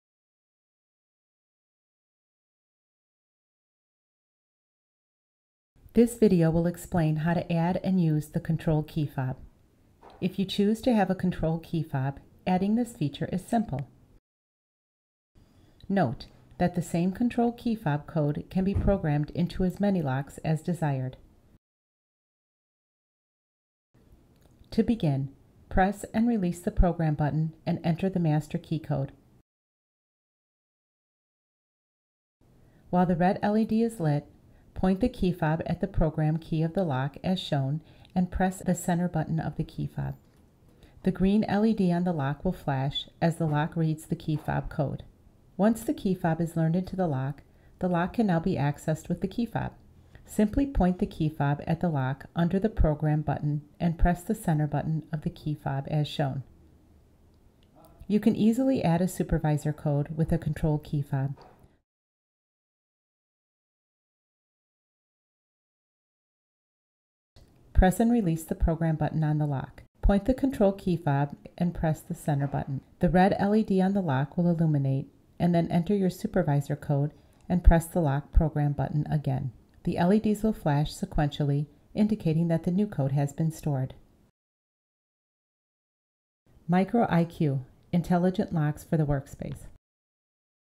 Adding a Control Key Fob
Learn how to add and use a control key fob with the microIQ lock. The control key fob will enable an administrator to access the lock without having to use the keypad.
The MicroIQ electronic locks are the perfect keyless locks to secure your lockers. MicroIQ locks fit directly into the existing double- D hole, providing a keyless replacement for mechanical locks. The easy-to-use keypad lock can be accessed with a keypad code and/or a key fob. The locker mode operates with one-time user codes.
Upcoming models feature NFC, wireless, and RFID badge card access alternatives.